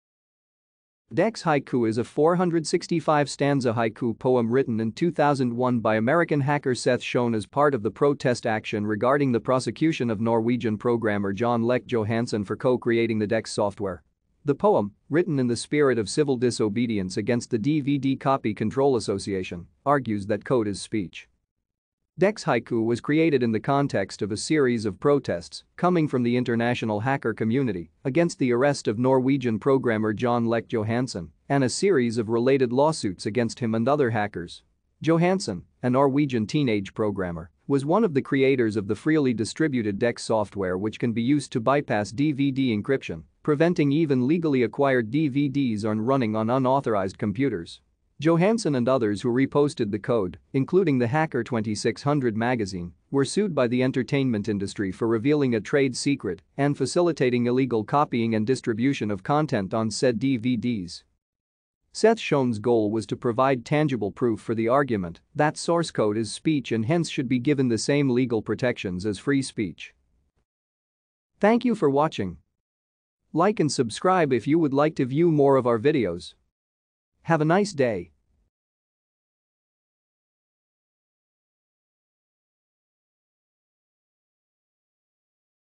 DeCSS Haiku
DeCSS haiku is a 465-stanza haiku poem written in 2001 by American hacker Seth Schoen as part of the protest action regarding the prosecution of Norwegian programmer Jon Lech Johansen for co-creating the DeCSS software. The poem, written in the spirit of civil disobedience against the DVD Copy Control Association, argues that "code is speech."